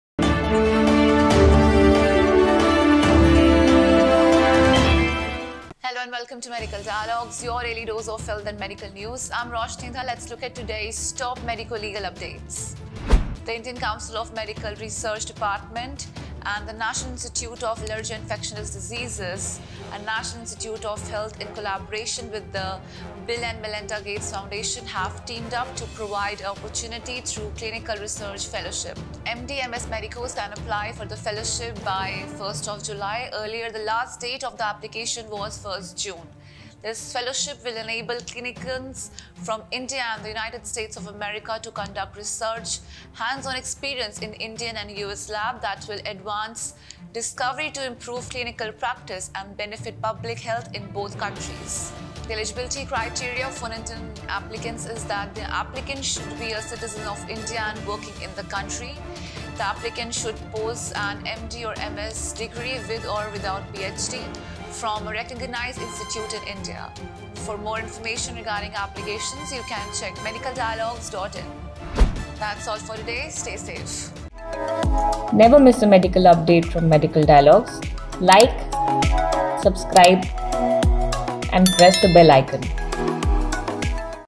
ICMR, Invites Applications For Clinical Research Fellowship Programme
The Indian Council of Medical Research (ICMR)/Department of Health Research (DHR) and the National Institute of Allergy and Infectious Diseases (NIAID)/ National Institutes of Health (NIH), in collaboration with  the Bill & Melinda Gates Foundation (BMGF), have teamed up to provide a opportunity through Clinical Research fellowship.